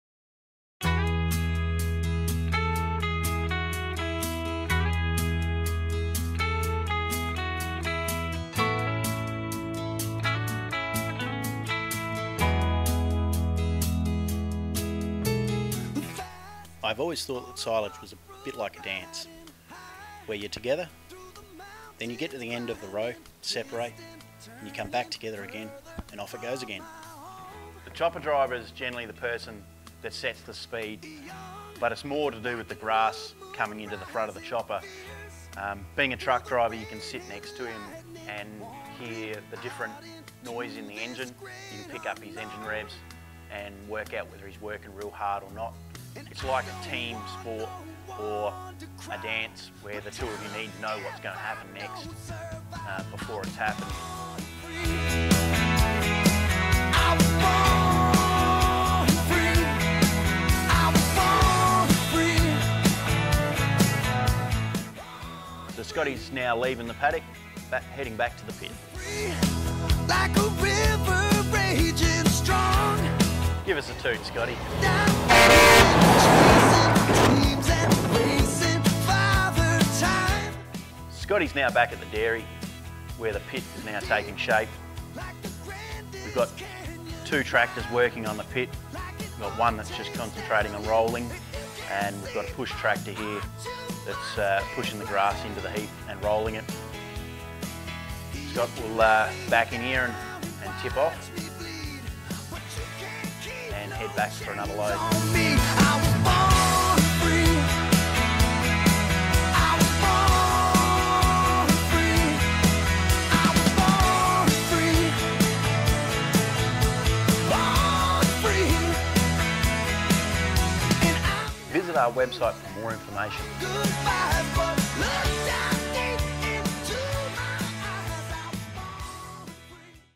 Mount Compass fertilisers Silage
Cutting silage on the Fleurieu Peninsula helping the farmers